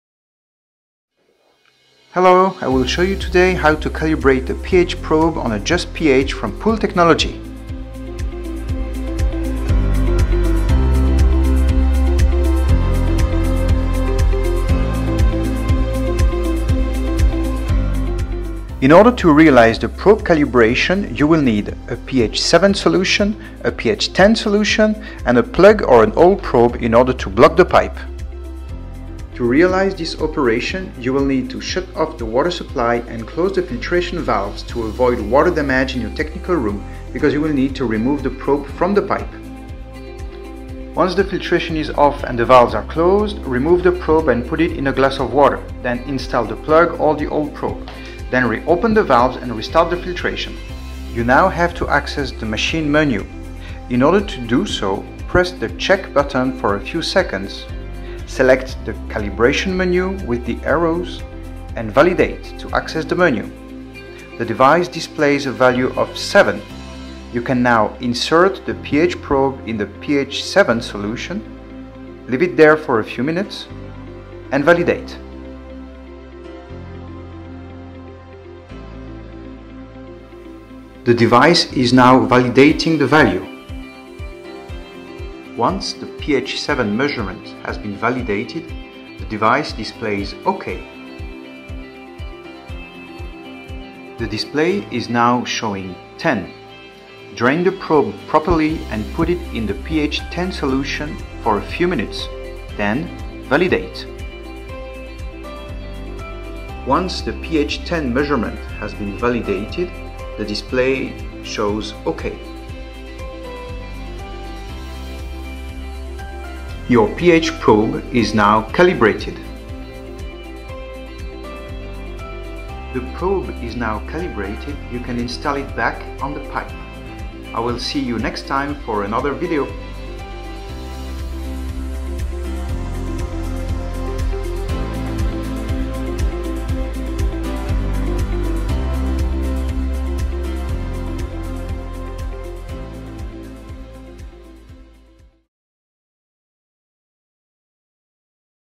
pH probe calibration - POOL TECHNOLOGIE
Calibration of the pH probe on a JUST pH POOL TECHNOLOGIE

This calibration is also compatible with the versions of "JUPHPP ..." devices.